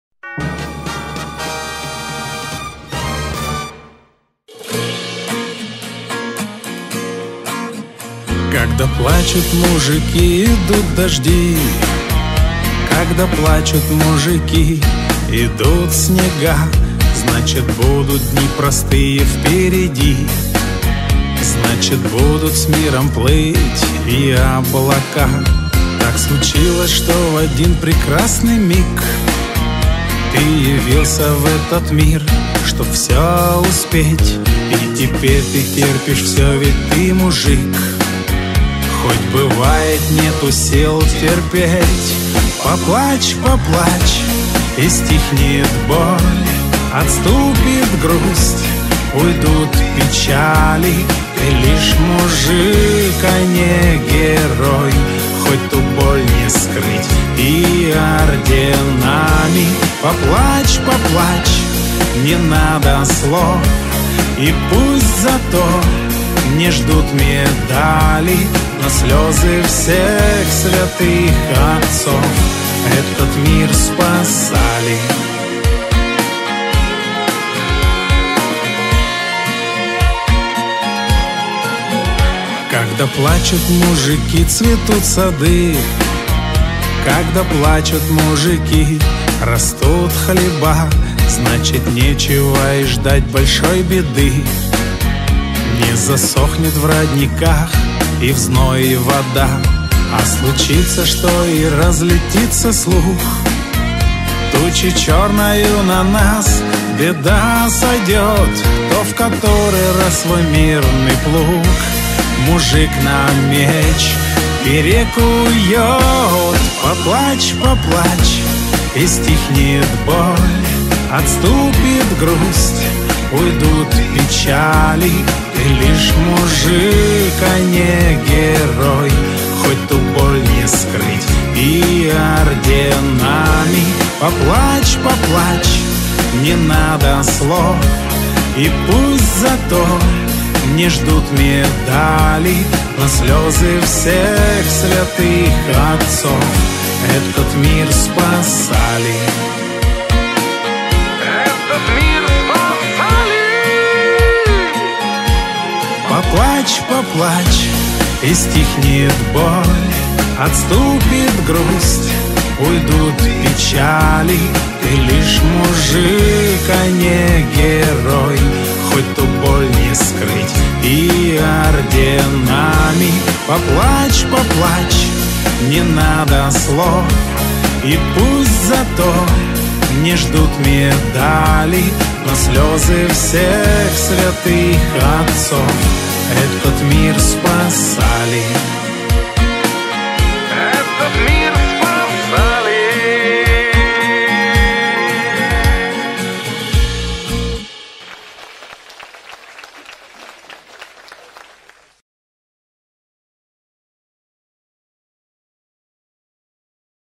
Фестиваль Спасти Жизнь. Save a Life . Hamburg 2015.
Павел Бургов на фестивале Save a Lefe. Hamburg